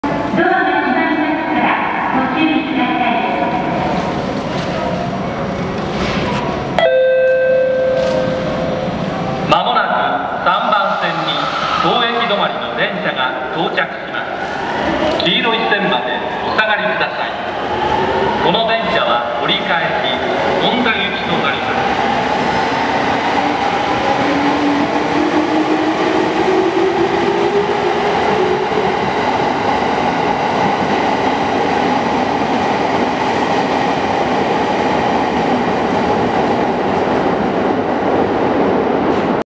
JR209系500番台発車
武蔵野線に転属した209系500番台。